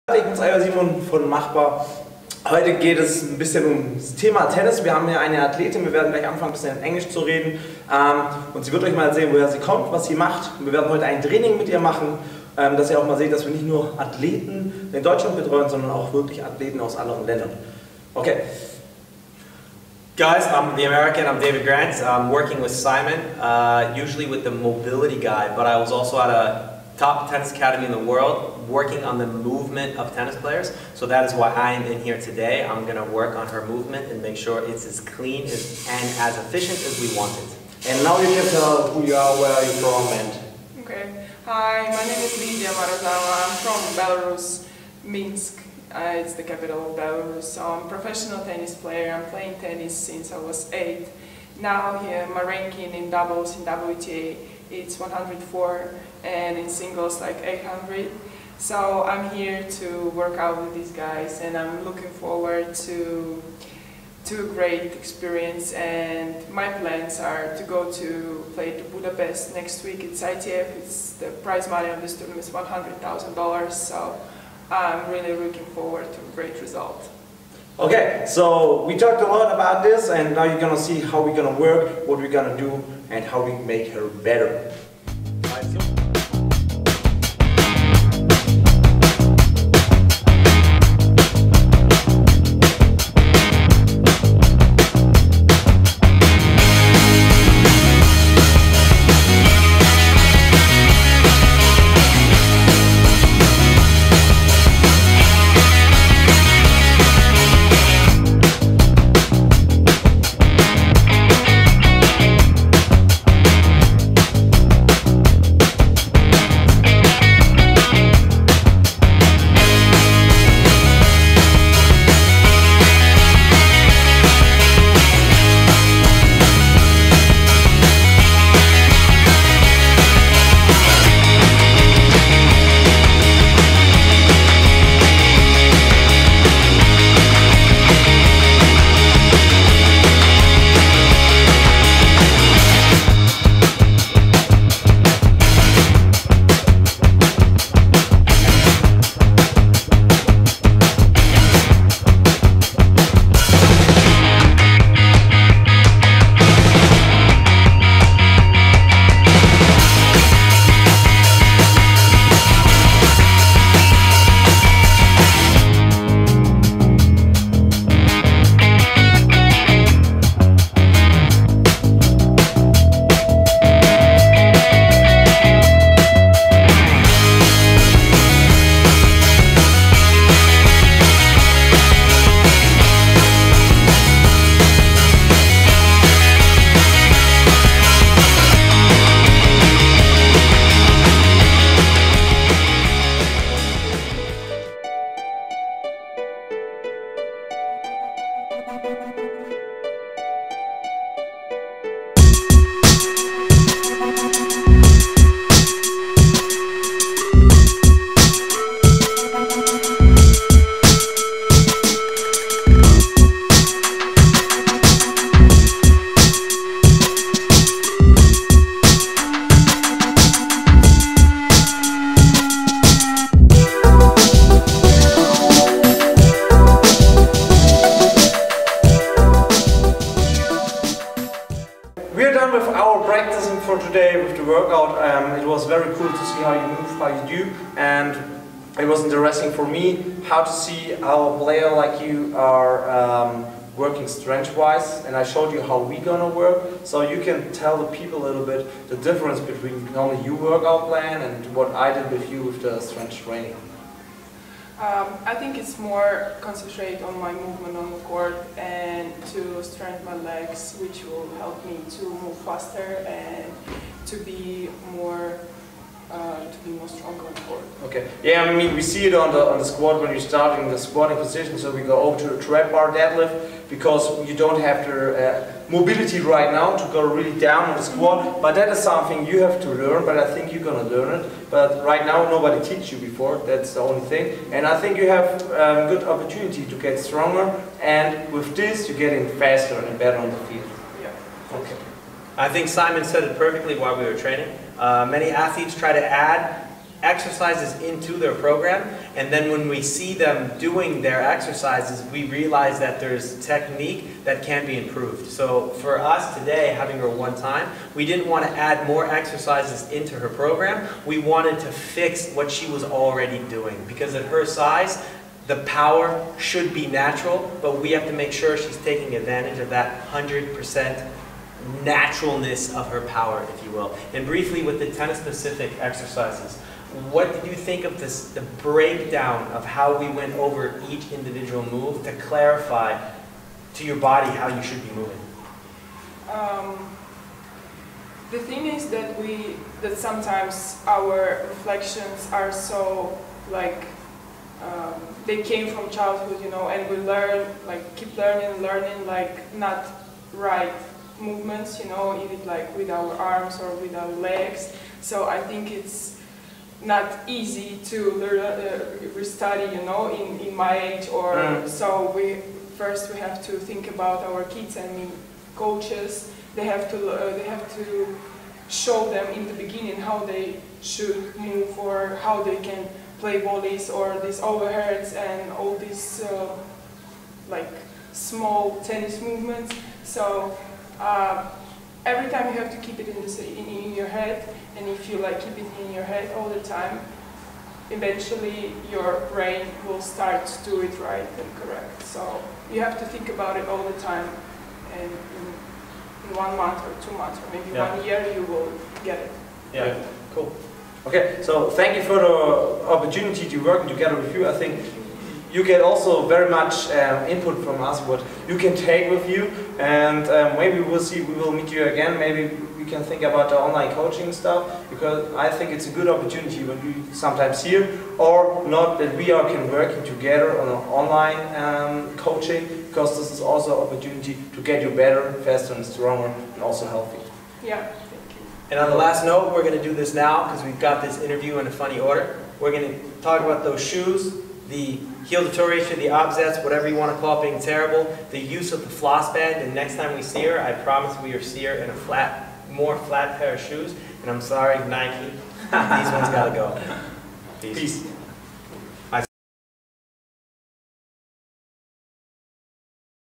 Impression Workout with Lida Marozava Tennis Player (Double Ranking 104) from Belarus
II don't Just train athletes from Germany, together with David Grant we prepared top 100 WTA doubles player Lida Marozava (Belarus) feor her next tournament!!

If she contintues to play like this she has a good chance to make Wimbeldon!!!

Tennis Pro from Belarus - Lida Marozava

Best Ranking Single - 495 (13/07/2015)
Current Doubles Ranking 104